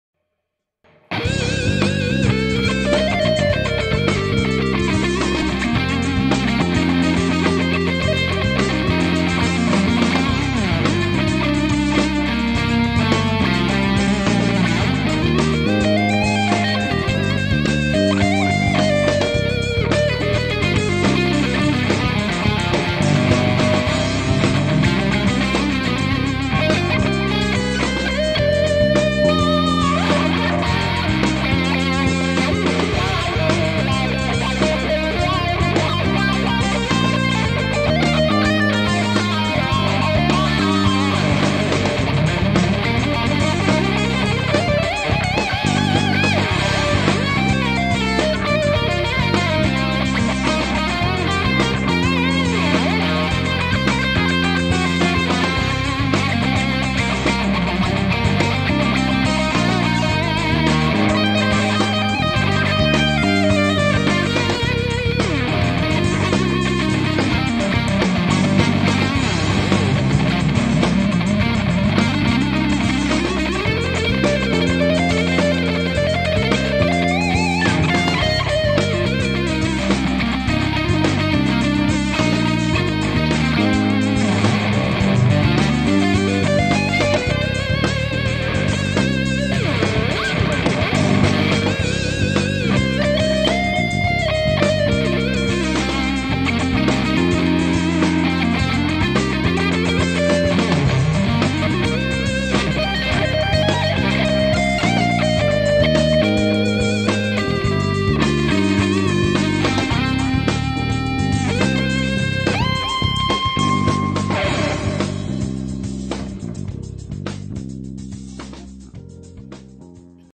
William - Fusion Jam II
William, Guitar Coach der The! Music School, beim "jammen".